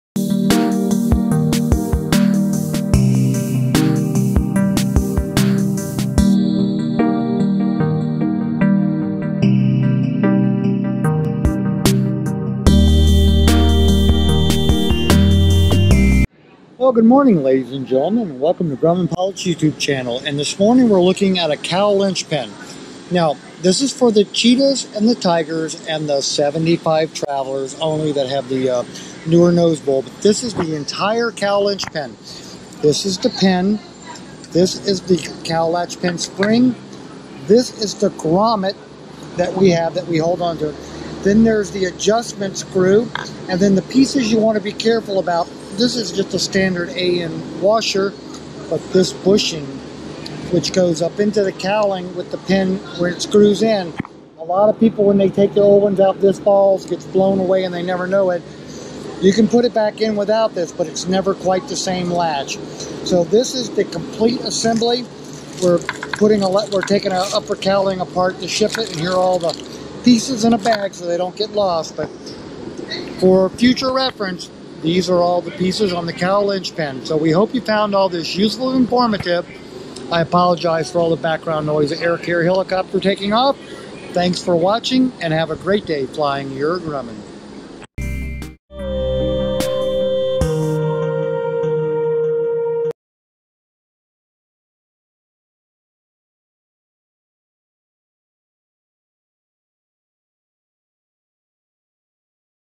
Cowling Latch Pin Assembly - Grumman Style
Cowling Latch Pin Assembly - Grumman Style:  Very quick look at the cowling (upper) latch pin assembly.  Video 975 in the Hit Parade.